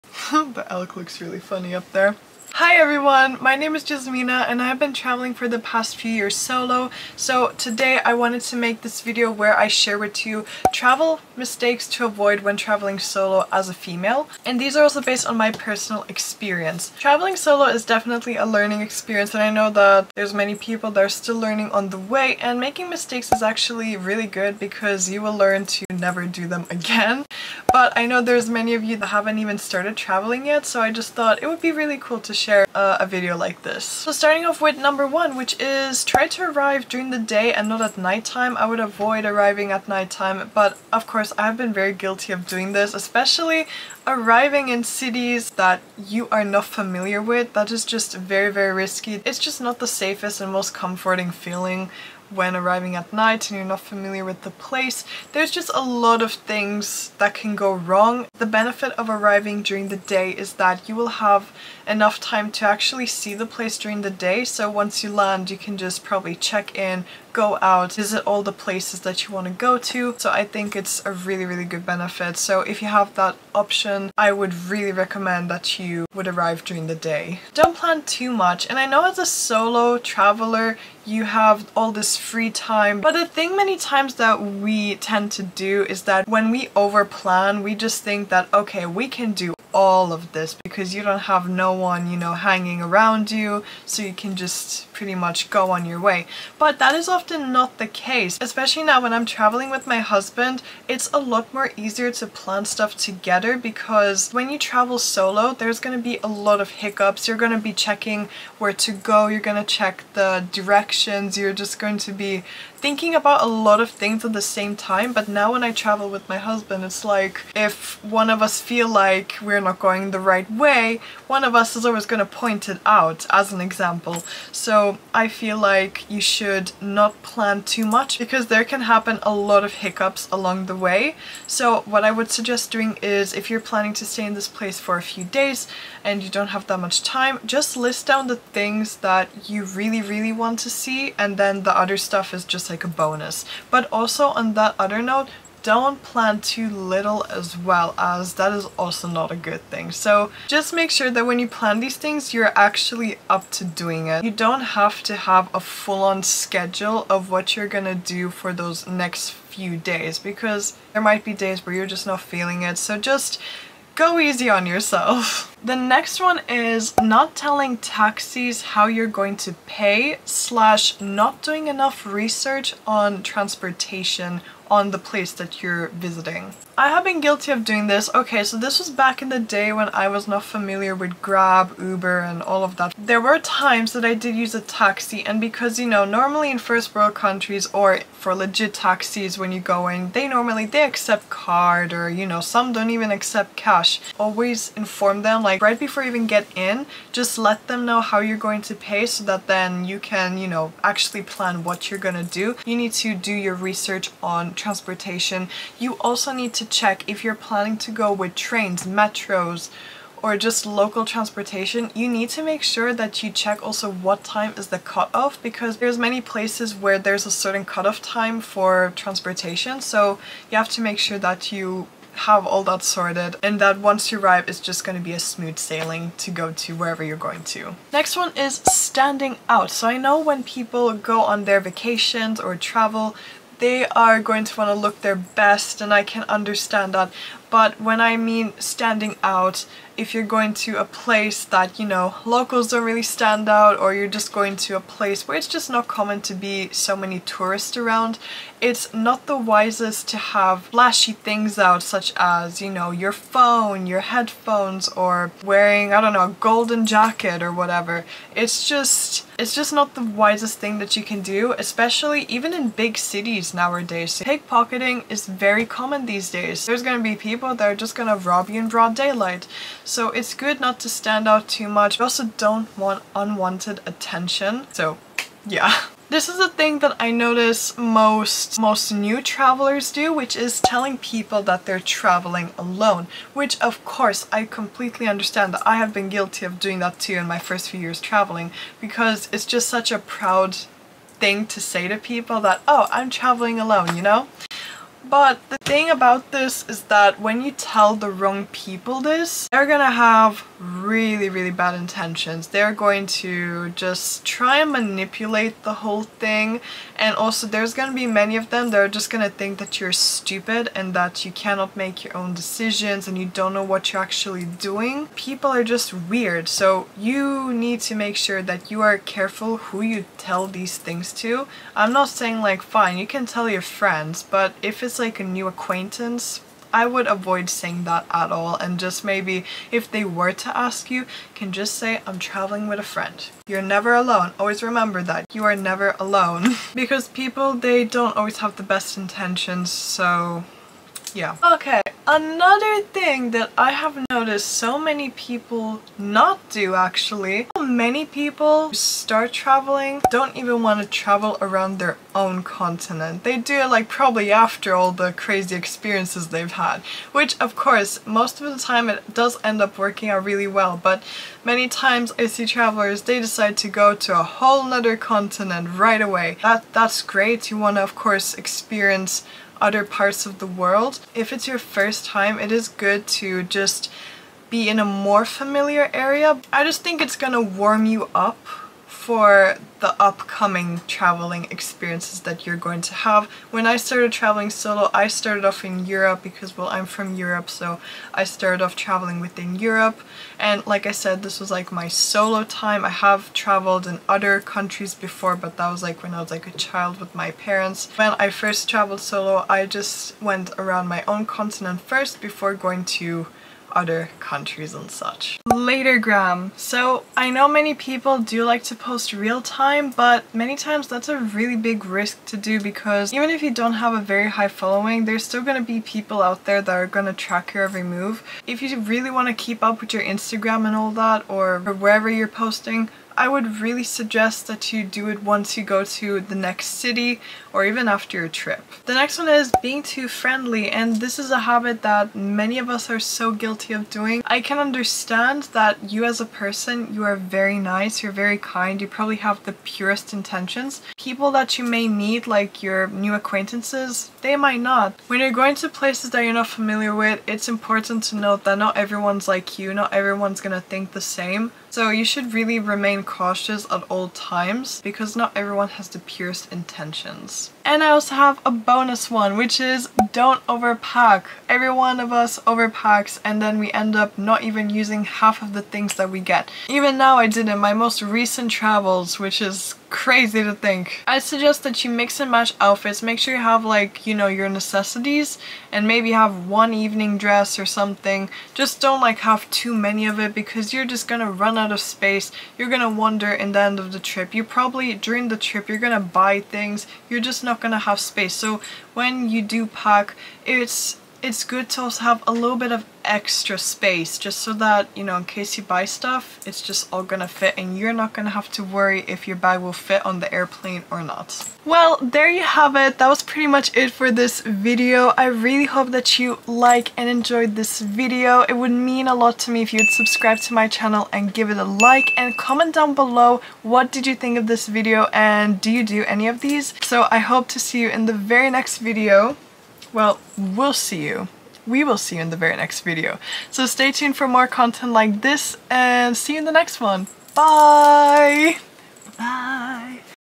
Things I Have Learned While Traveling Solo - Avoid These As A Woman Traveler
These are some tips i thought id share, hope you find these helpful ☺️